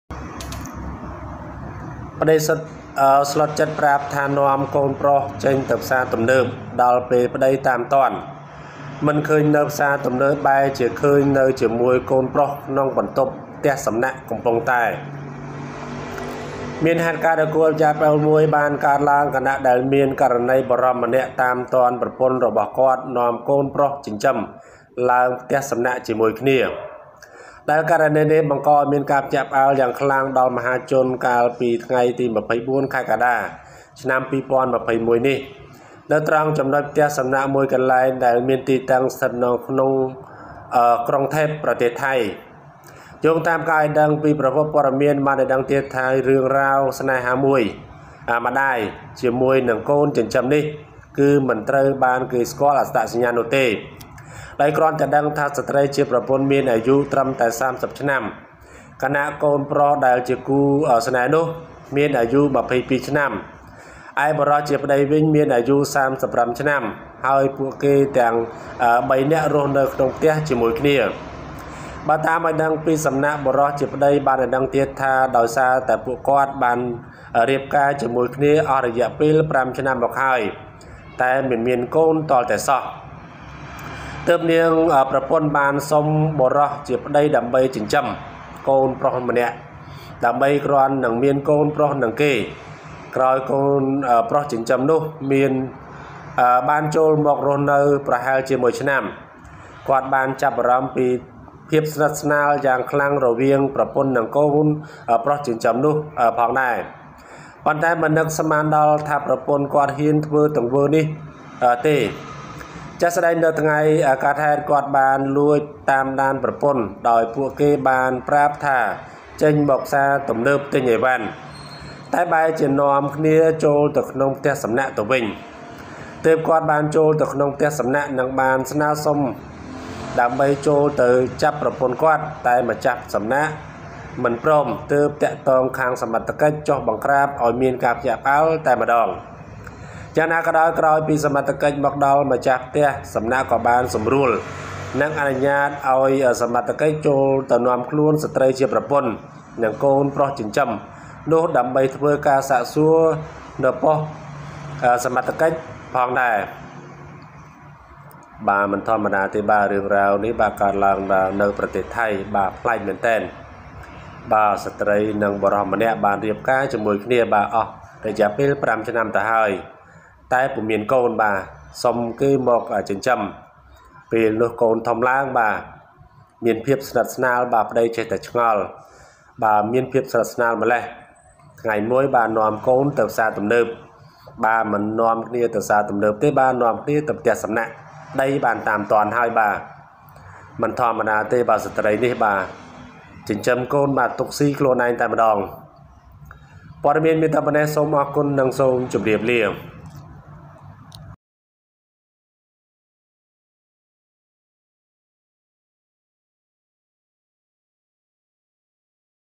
ប្តីស្លុតចិត្ត! ប្រាប់ប្តីថា នាំកូនប្រុសចេញទៅផ្សារទំនើប ដល់ពេលប្តីតាមទា ន់ មិនឃើញនៅផ្សារទំនើប
Husband shocked!  Tells her husband to take her son to the mall

ប្តីស្លុតចិត្ត! ប្រាប់ប្តីថា នាំកូនប្រុសចេញទៅផ្សារទំនើប ដល់ពេលប្តីតាមទា ន់ មិនឃើញនៅផ្សារទំនើប បែរជាឃើញនៅជាមួយកូនប្រុសក្នុងបន្ទប់ផ្ទះ សំ ណា ក់ កំពុងតែ